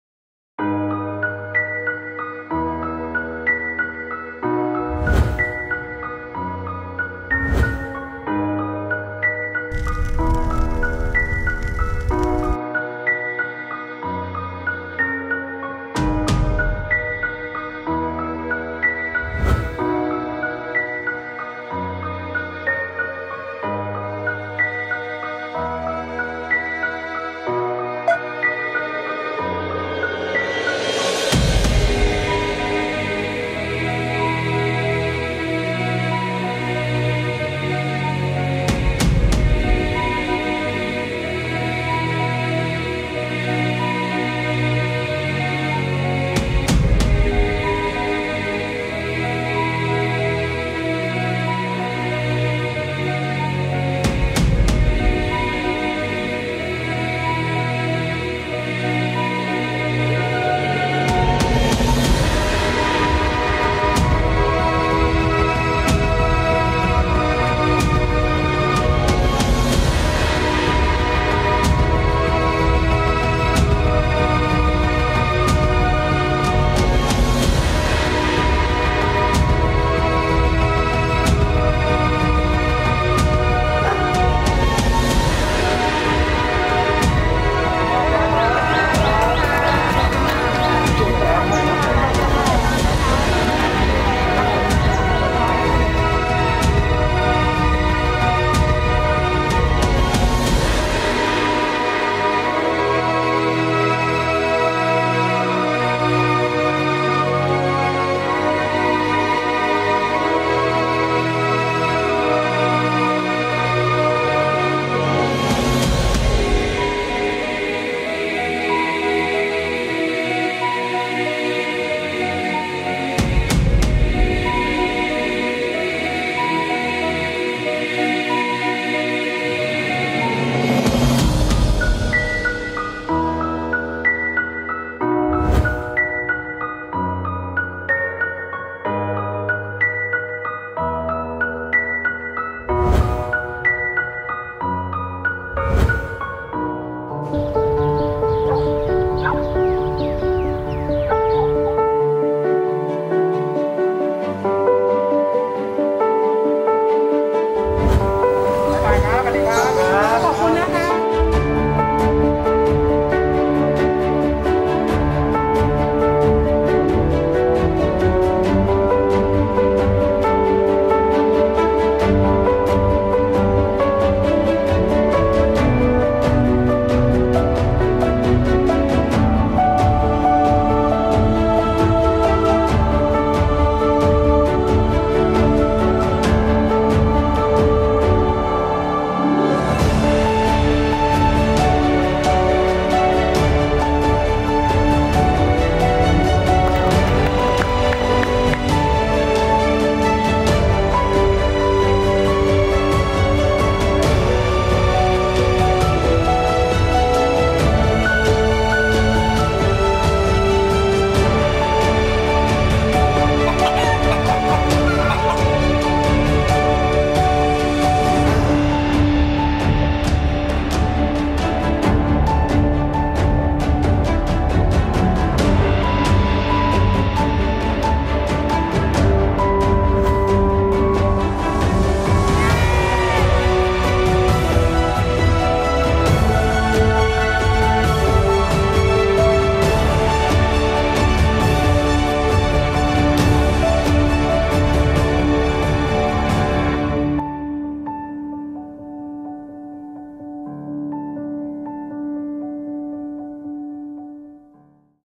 GWM : GO WITH ME, GO TOGETHER รวมหัวใจพร้อมก้าวผ่านทุกวิกฤตไปด้วยกัน
เกรท วอลล์ มอเตอร์ ร่วมยืนหยัดเคียงข้างคนไทยในสถานการณ์การแพร่ระบาดของไวรัสโควิด-19 ผ่านโครงการ “GWM : GO WITH ME, GO TOGETHER”  จับมือ Food For Fighters พร้อมสนับสนุนเงินทุน 1 ล้านบาท รถยนต์ All New HAVAL H6 Hybrid SUV ชวนทำกิจกรรมเพื่อสังคม เพื่อร่วมเป็นส่วนหนึ่งในการช่วยเหลือให้คนไทยก้าวผ่านช่วงเวลานี้ไปด้วยกัน